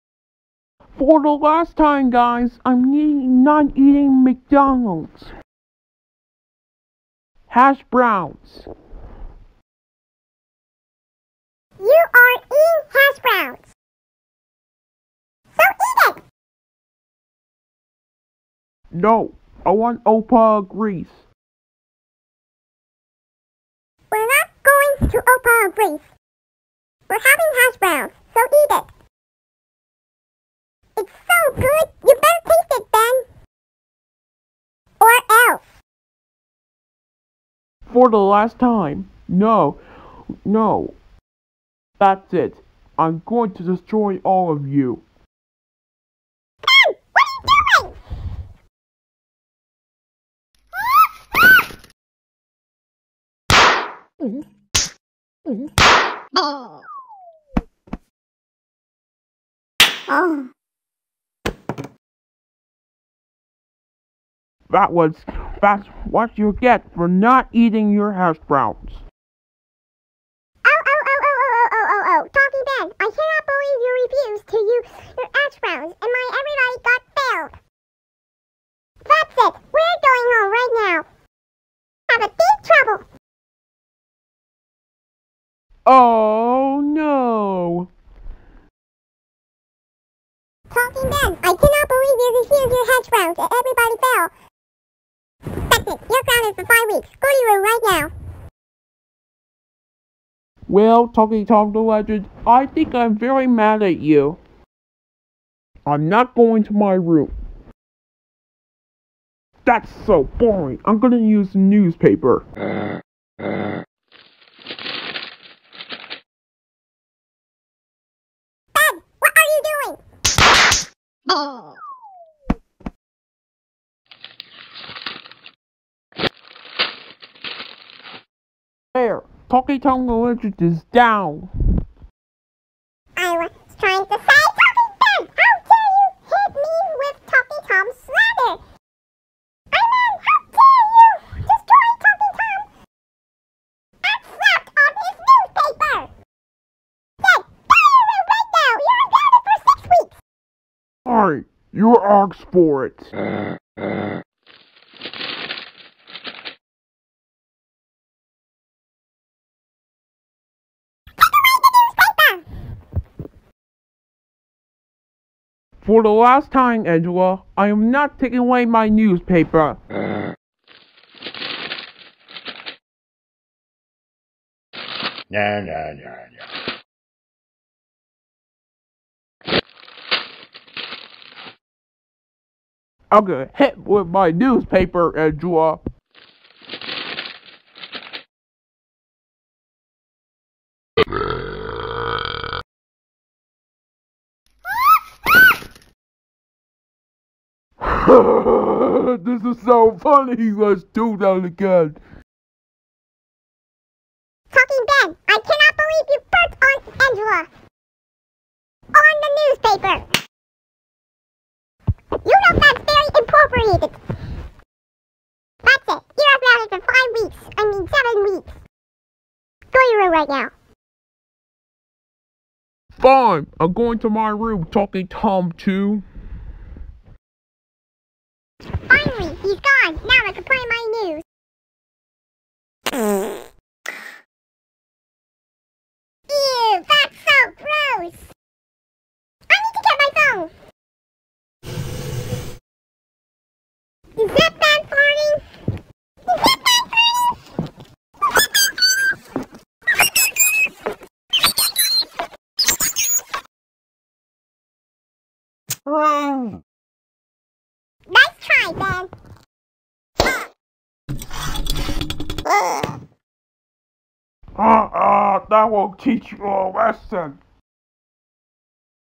Talking Ben Refuses To Eat His Hasbrowns
This is what happens when Ben refuses To Eat His Hasbrowns